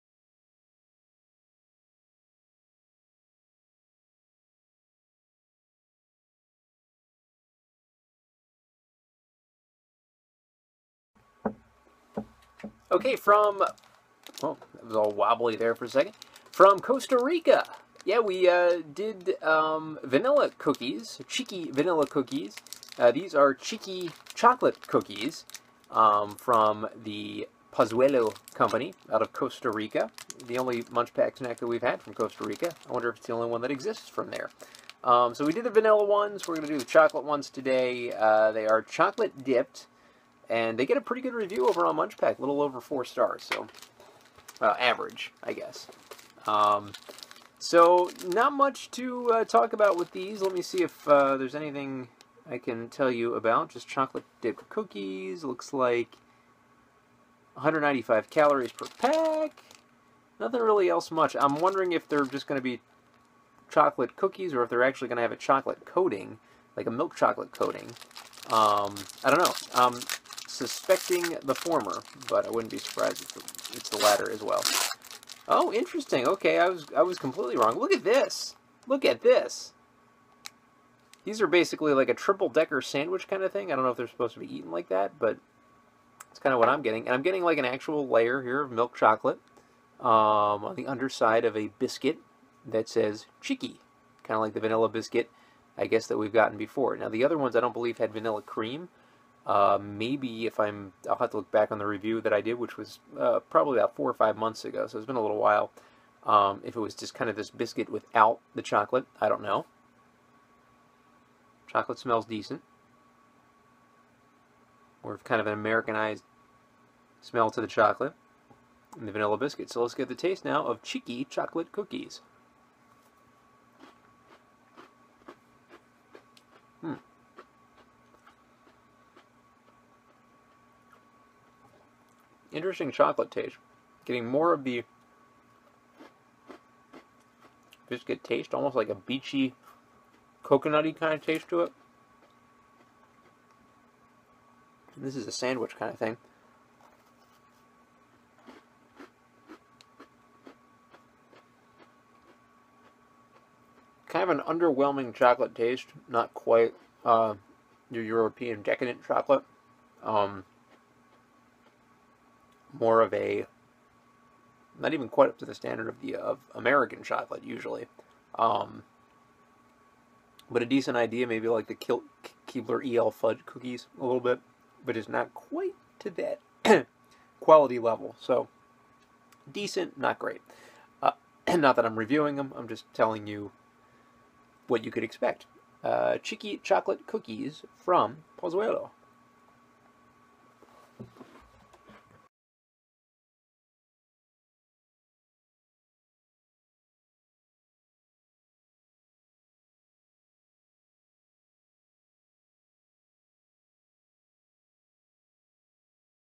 Munchpak Snack Review: Chiky Chocolate Cookies
From Costa Rica.  Basically the vanilla wafers with a dull, milk chocolate coating.